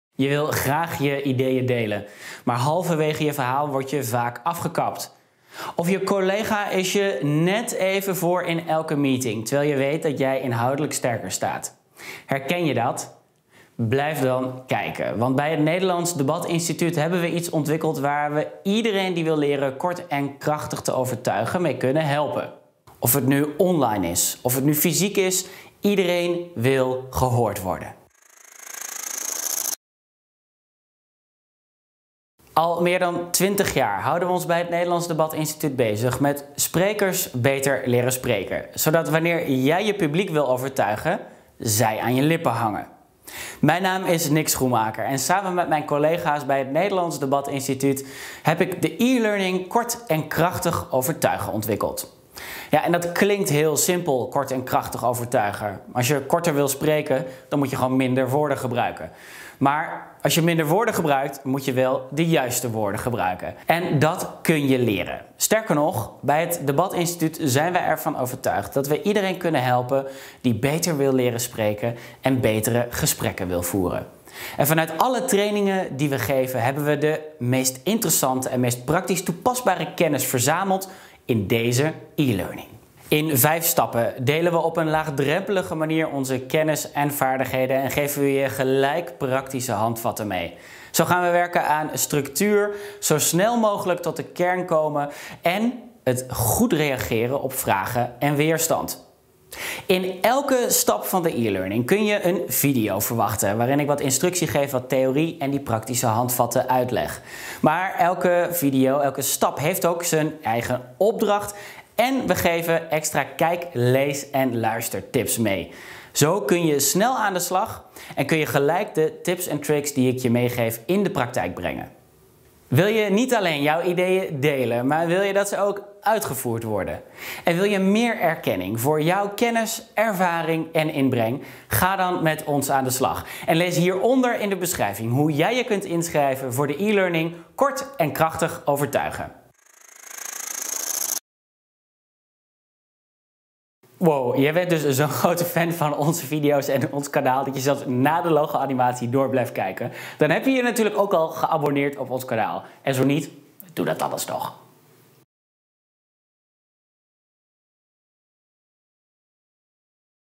Kort en krachtig overtuigen? Leer het online in 6 dagen!
Denk jij regelmatig ‘had ik dat nou maar gezegd’ na afloop van een vergadering of gesprek met een klant? Of heb je het gevoel dat je niet gehoord wordt tijdens de momenten waarin je jouw ideeën naar voren brengt? Als dat herkenbaar is, is onze e-learning kort en krachtig overtuigen iets voor jou. We geven je praktische handvatten om die momenten wél aan te grijpen en overtuigend te spreken in allerlei werksituaties, online en offline. Bekijk deze video voor meer informatie.